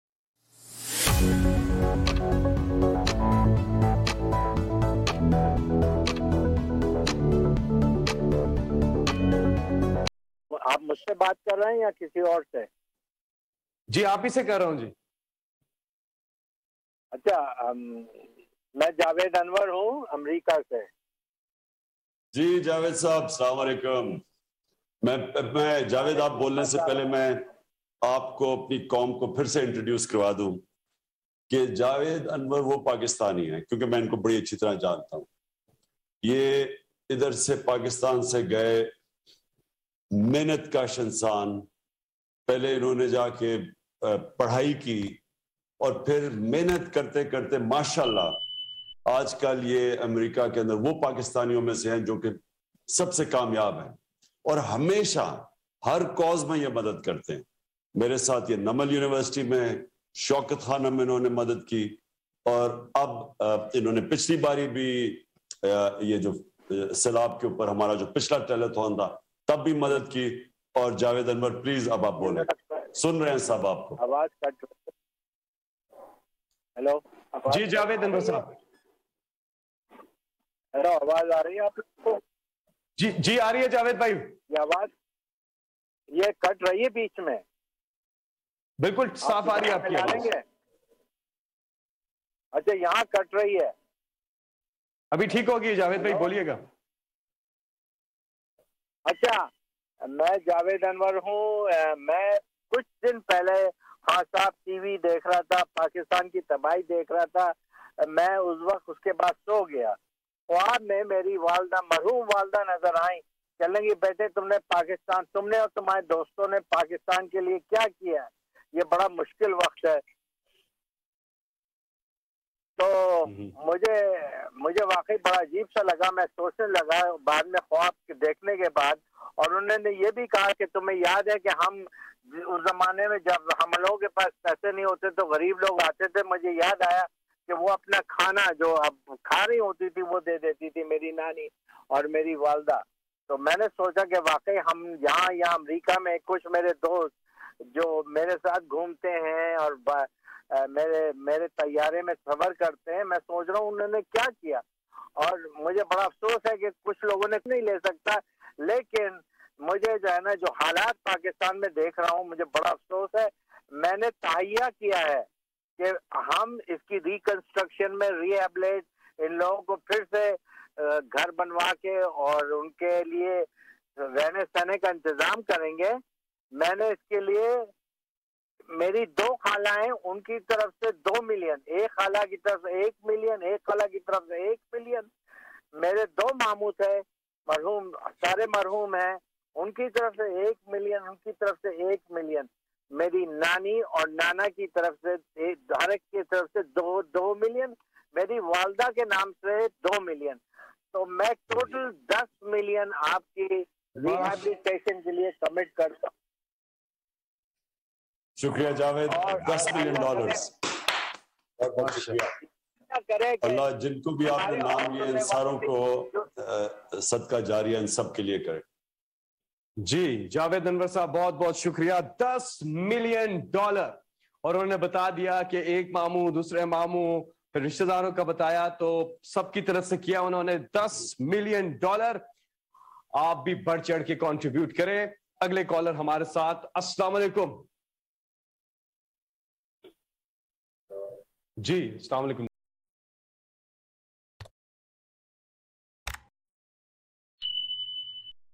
Javed Anwar Donate 1crore dollars to flood victims | Imran khan telethon | khabar ki dunya
Credit: BOL News